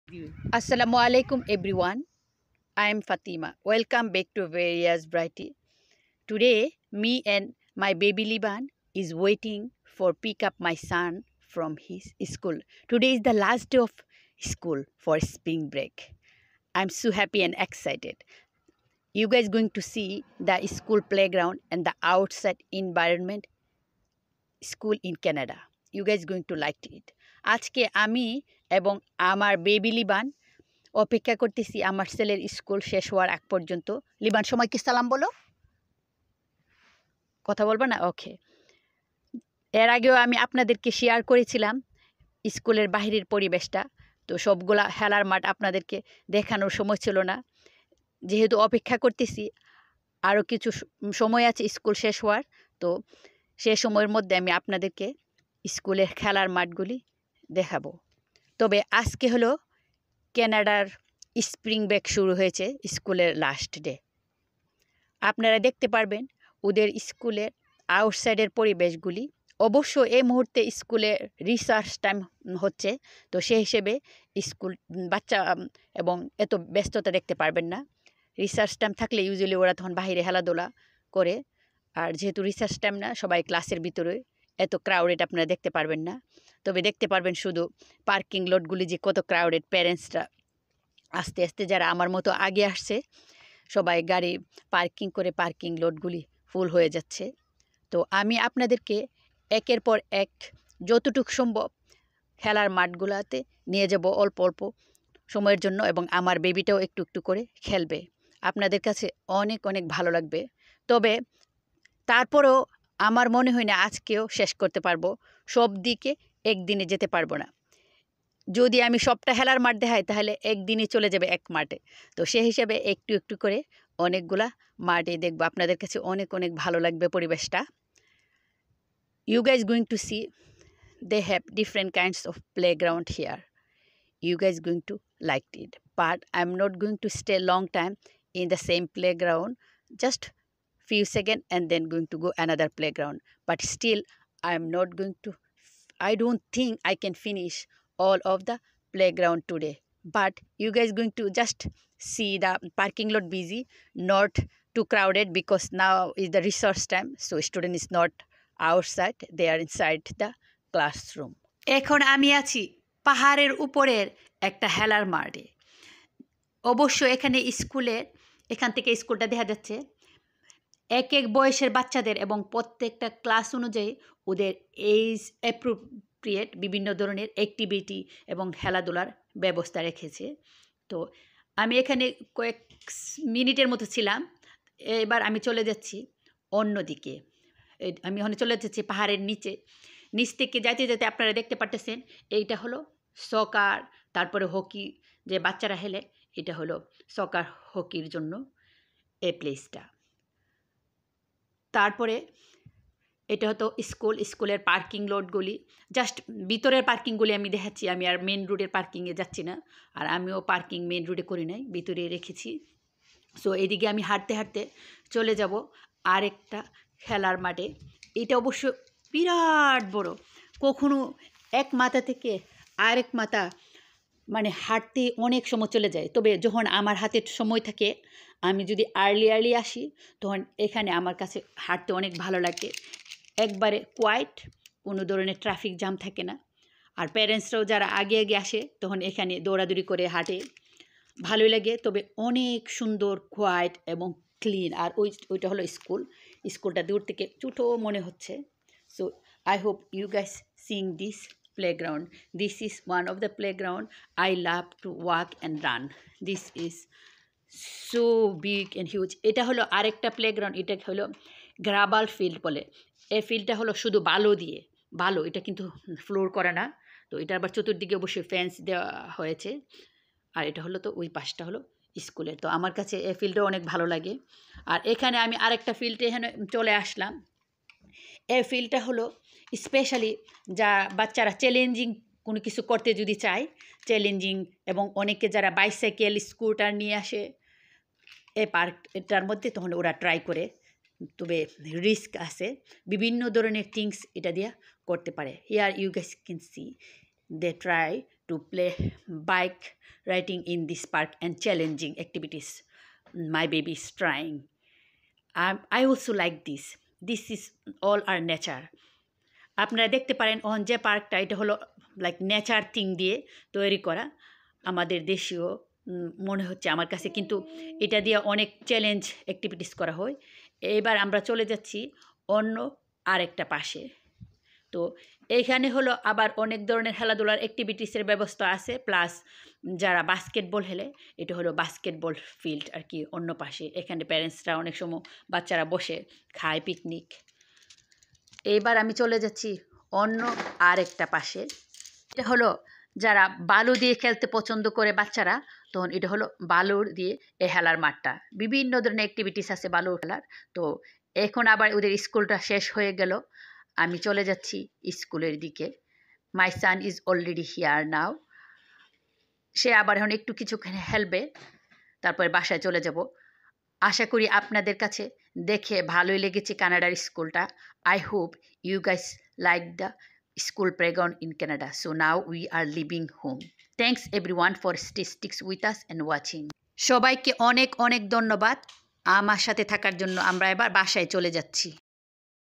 School Play Ground Tour in Canada! কানাডার স্কুলের খেলার মাঠ গুলো দেখতে কেমন!
School Play Ground  Tour in Canada! কানাডার স্কুলের খেলার মাঠ গুলো দেখতে কেমন! I am here to. pick up my son from his school!